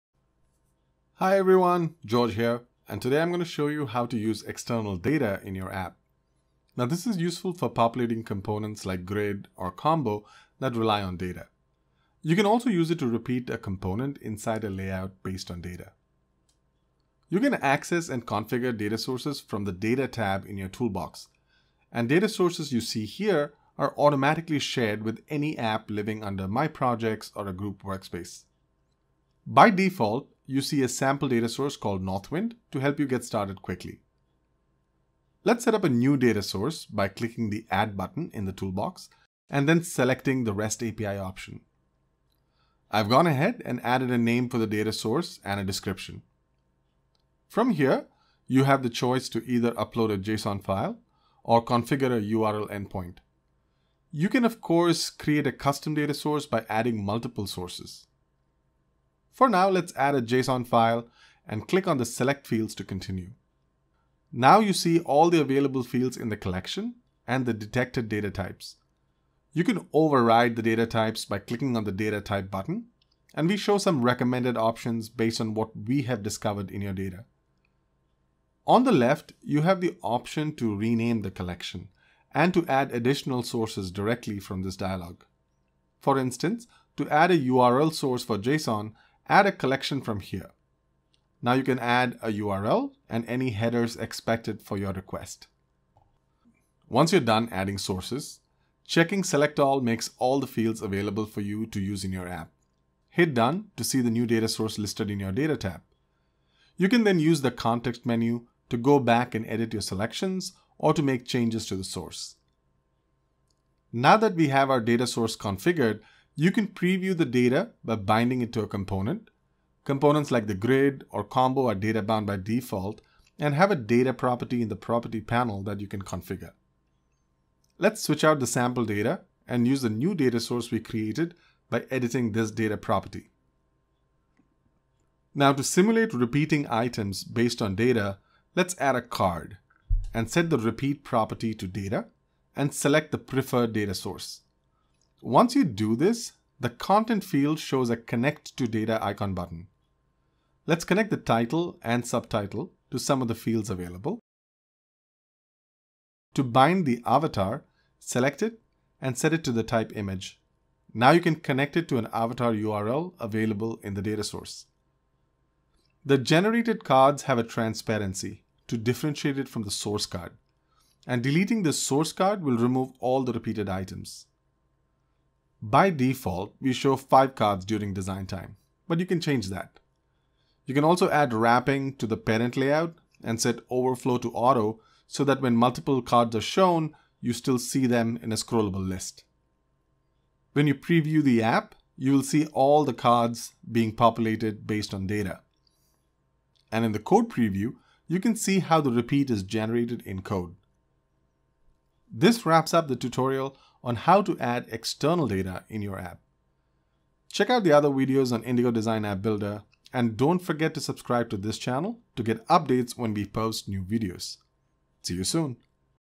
How to Use External Data in Your App Easy and Fast
A quick tutorial to help you learn how to use external data in your app fast and easily. This guide is useful for populating components like Grid or combo that rely on data. You can also use it to repeat a component inside a layout based on data.

This video will show you how to:

1. Create a New Data Source
2. Upload JSON or URL endpoint
3. Edit data source
4. Repeat items based on data

Don't forget to check out the other videos on the Indigo.Design app builder.